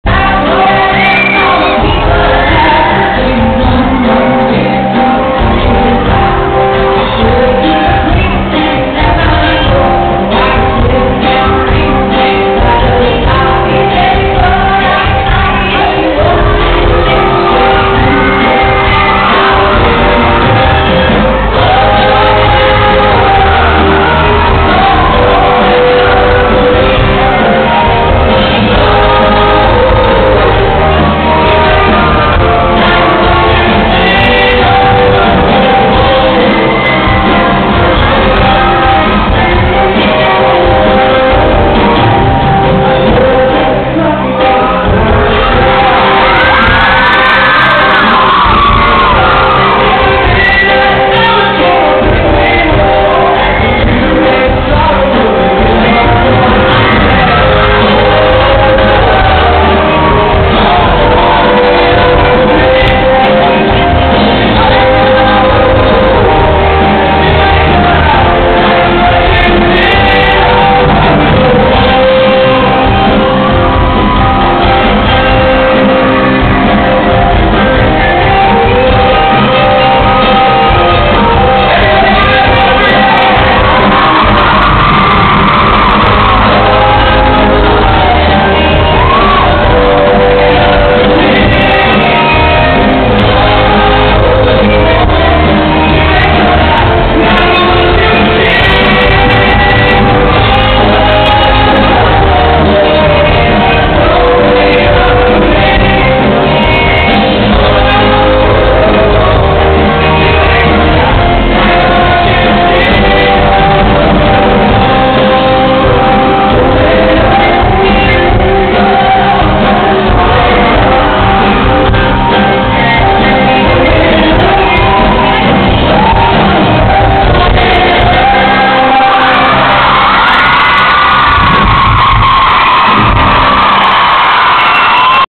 Diane Birch and special guest Travis from WTK 1-13-10
Travis from We The Kings was a special guest, performing with Diane Birch.